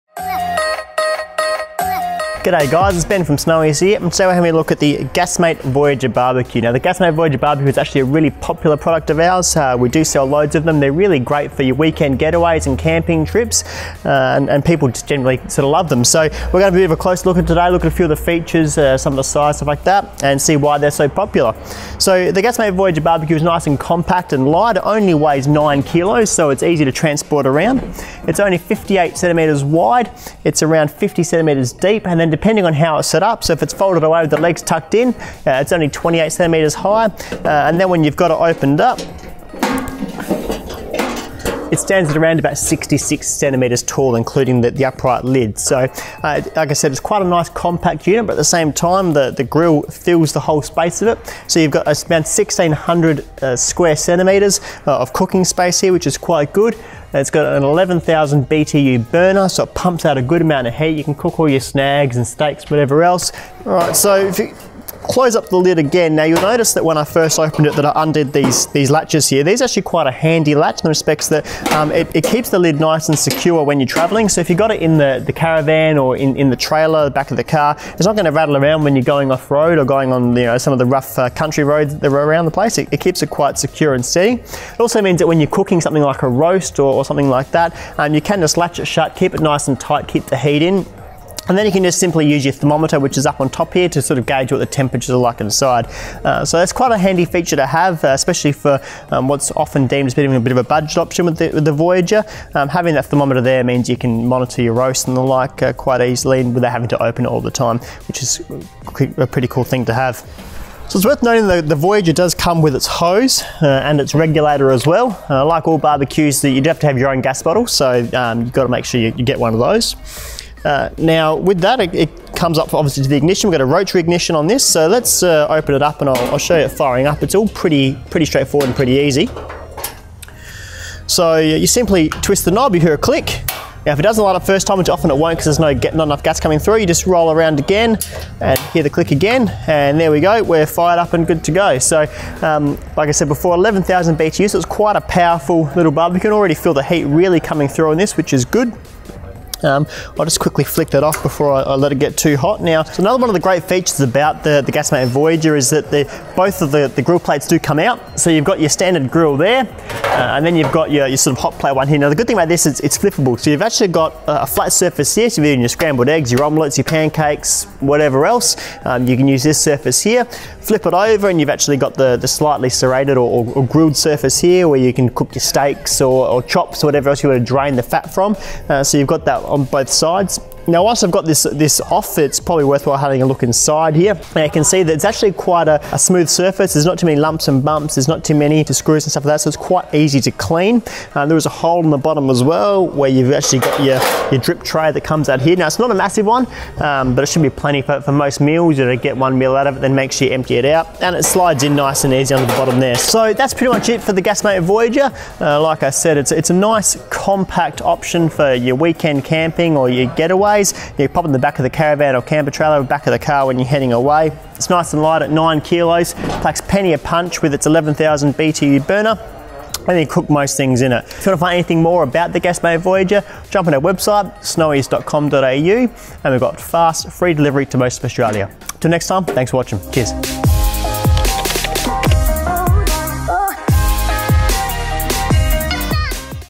Gasmate Voyager Portable BBQ
Whether it’s some delicious pancakes, bacon and eggs, or chops and sausages – the Voyager BBQ from Gasmate is the compact choice for cooking on your next adventure.

With a rotary ignition for easy lighting, a grill and cast-iron plate for cooking, a roasting hood with temperature gauge, and a POL hose and regulator included – you’ve got all the features you need for a roadside barbeque, from one convenient little unit.

As this portable unit is such a hot product, we’ve got our gear guru Ben to fire one up for us in store to show you how it works. He also runs you through all of the features so you can get a good look at what this unit is all about.

0:00 - Intro
0:24 - Weight & dimensions
0:56 - Burner BTU
1:06 - Latches
1:29 - Thermometer
1:50 - Hose and regulator
2:10 - Igniting the BBQ
2:33 - Grill plates
3:01 - Inside the BBQ
3:11 - Drip tray